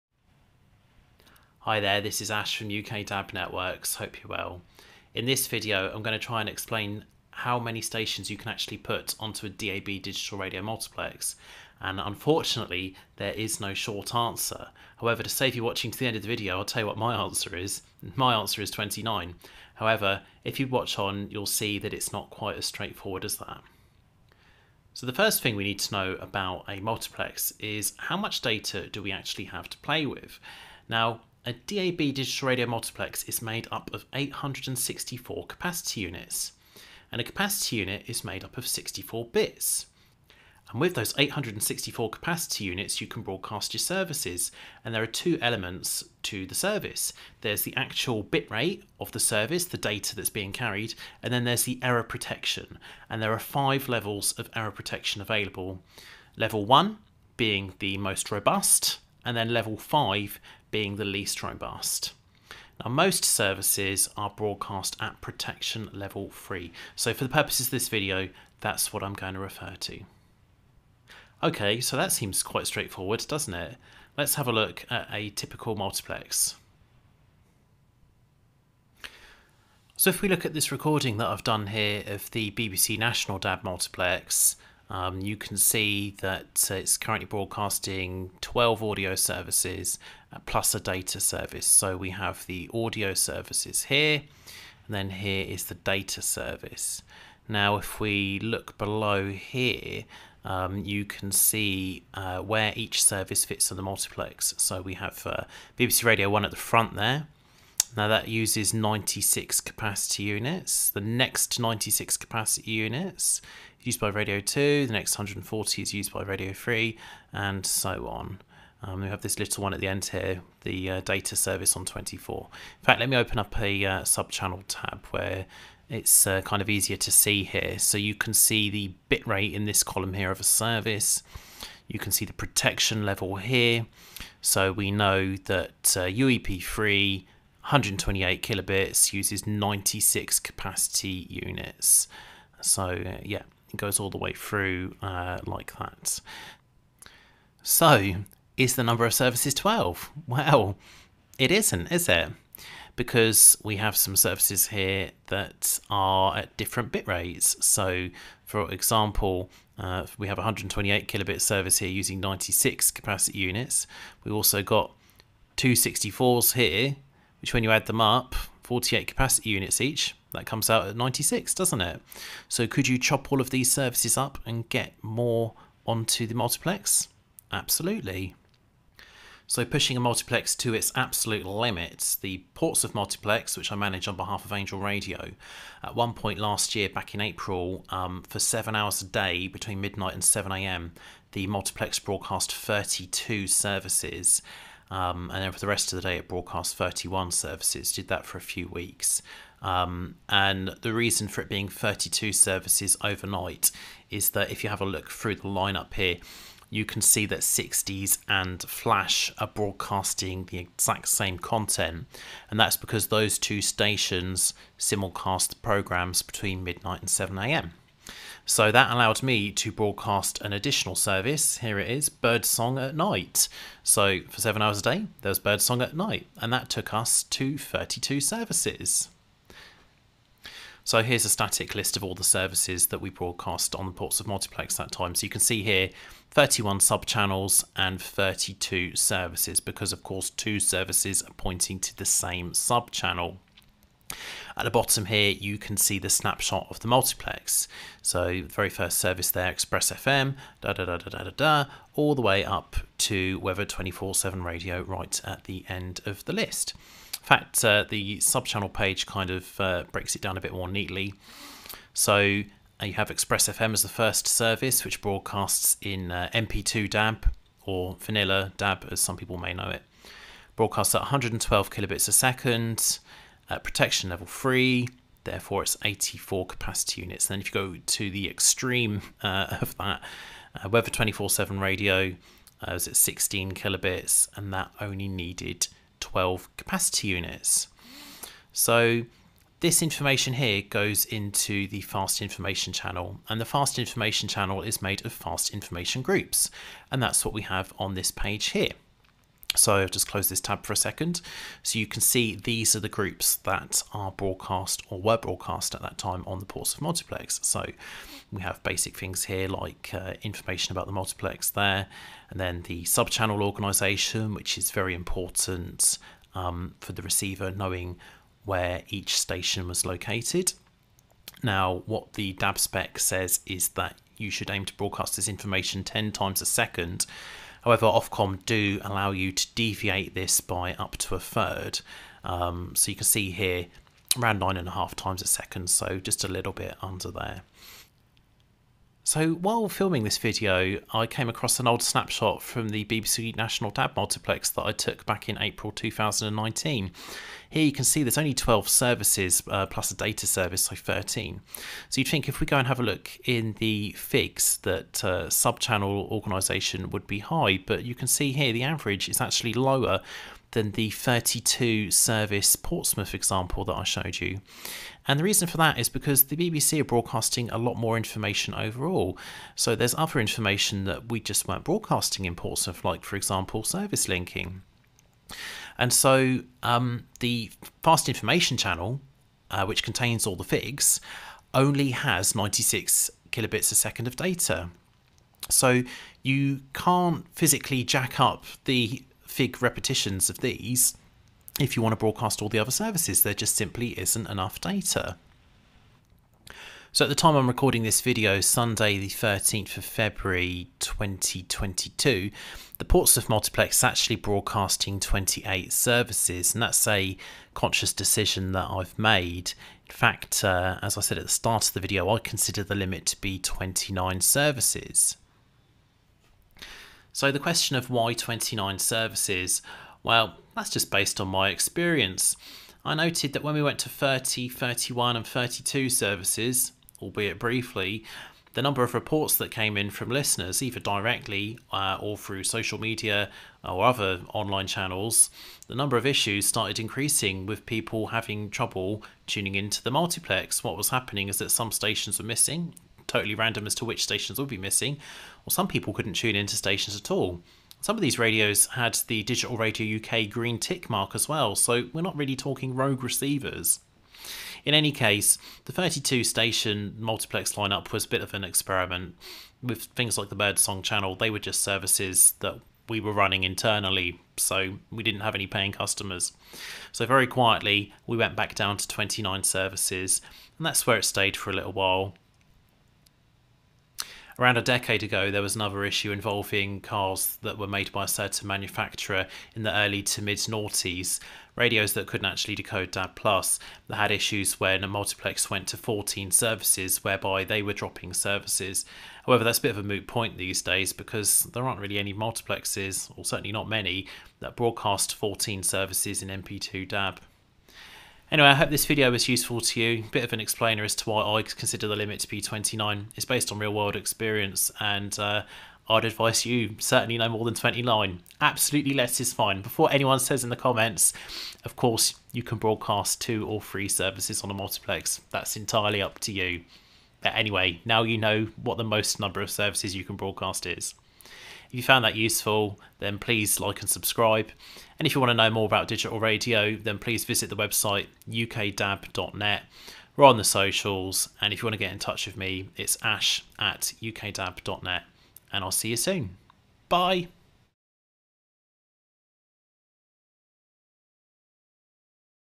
How many stations can you broadcast on a DAB+ digital radio multiplex? 📻
In this video, I explain how many stations can be broadcast on a DAB+ digital radio multiplex.

The short answer is that there are many factors at play which limit the number of services on a multiplex, but for me the limit is 29 (although less, is of course, fine).

If you want help to broadcast your radio station on DAB digital radio, or maybe you want to operate your own small scale DAB multiplex, please get in touch